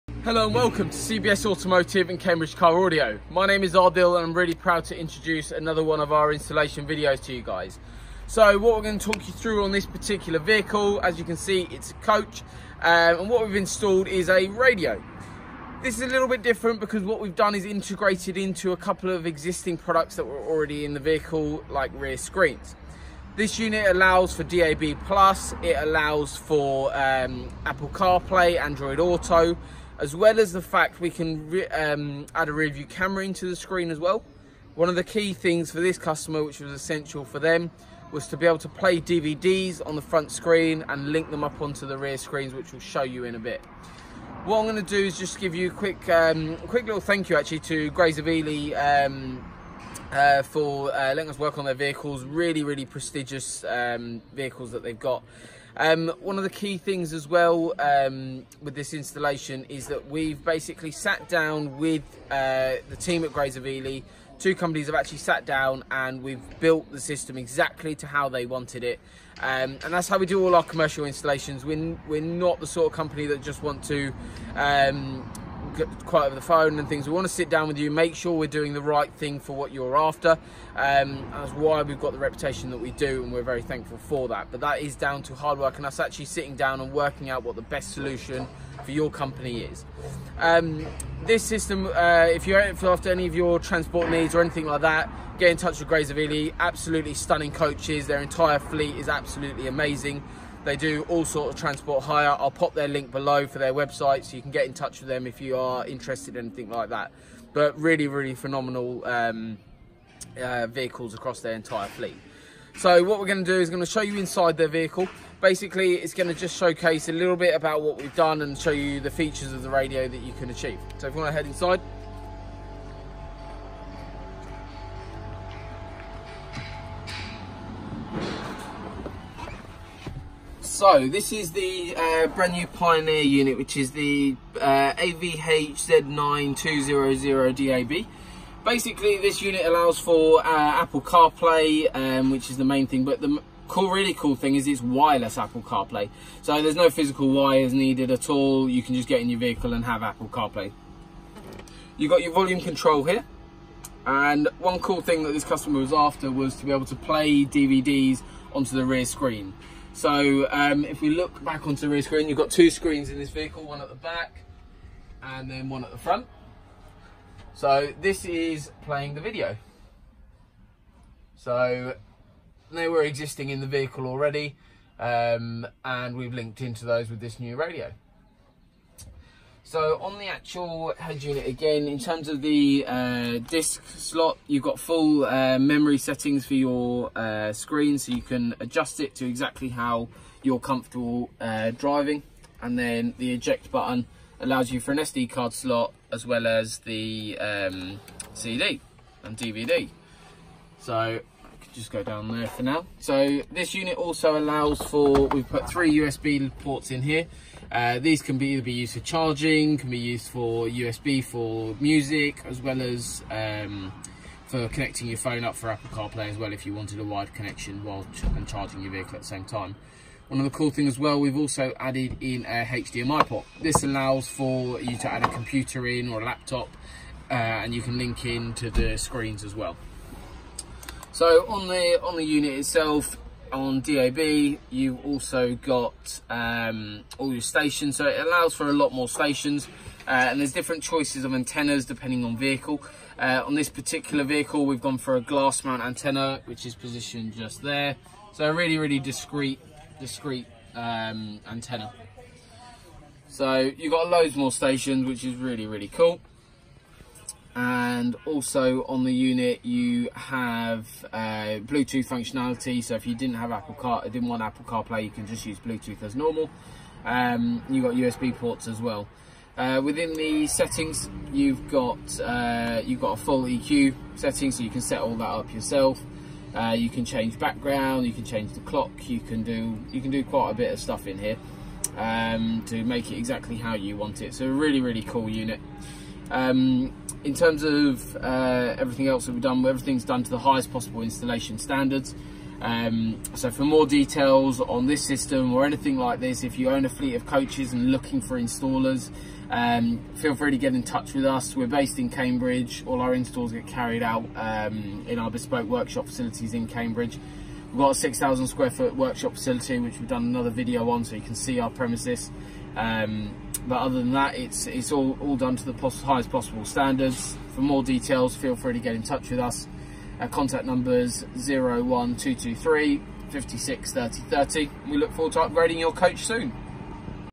Pioneer AVH-Z9200DAB - Greys of Ely Coach - CBS Automotive - Subscribe Now!
Flagship Wi-Fi enabled high-end multimedia entertainment AV system with a large 7-inch 24 bit True Colour Clear Type Resistive Multi-touchscreen. Enjoy Apple CarPlay wireless, Android Auto, Waze, Bluetooth and DAB+ Digital Radio (Digital Radio Tick Mark approved). Dual USB, HDMI and Dual camera input is also included, as well as superior audio and video performance.